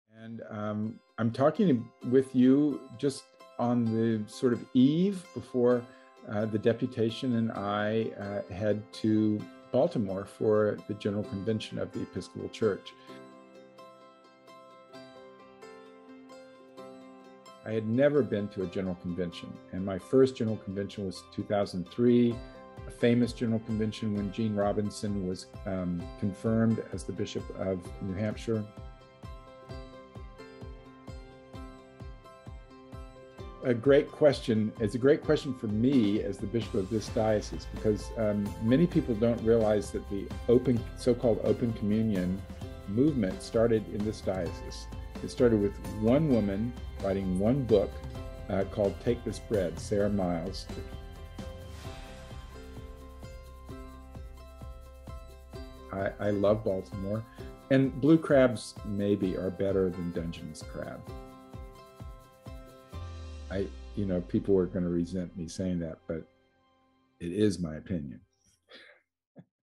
DioCal at GC80 - Trailer Ep.1
Preview to DioCal at GC80 Episode 1, where The communications working group has a conversation with Bishop Marc Andrus